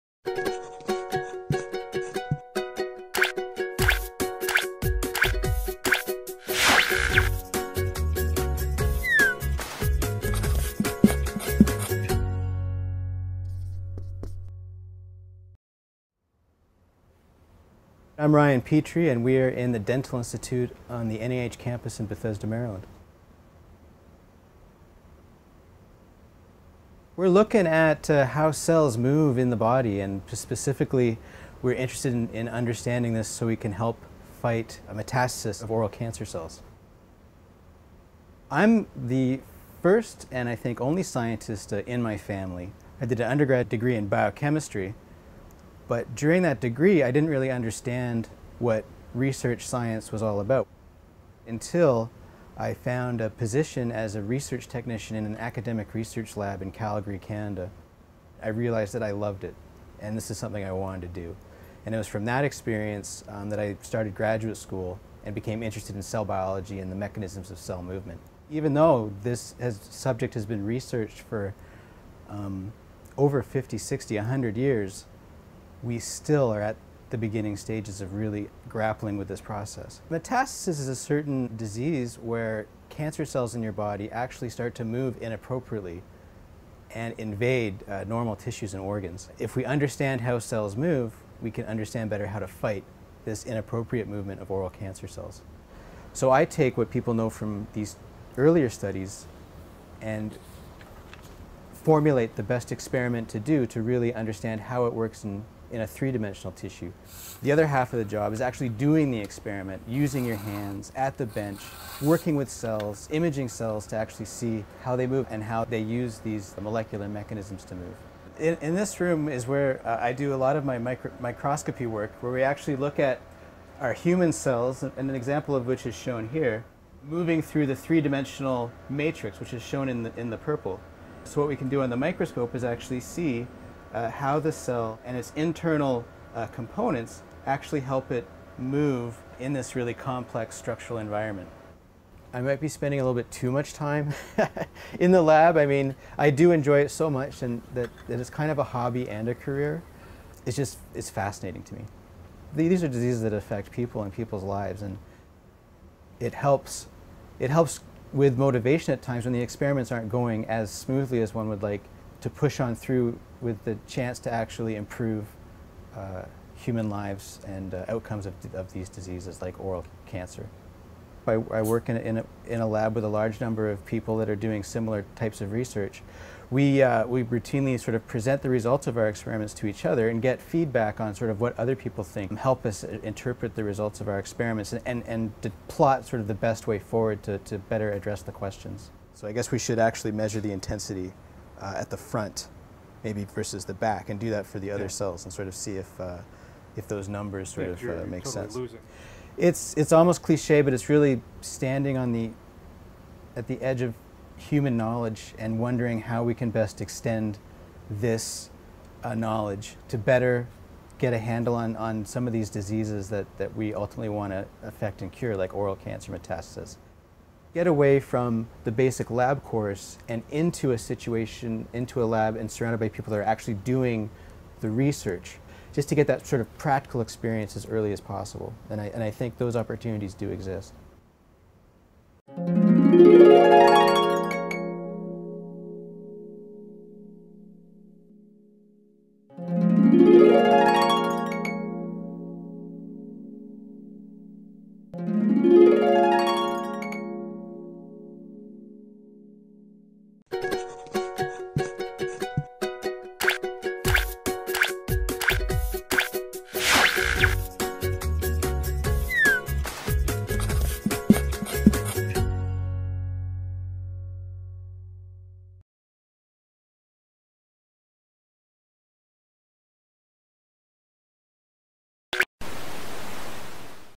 Searching for a Career? Explore Medical Research
Meet Dr. Ryan Petrie, a medical researcher in the National Institute of Dental & Craniofacial Research at the National Institutes of Health. Dr. petrie studies the movement of cells and is trying to find a cure for oral cancer.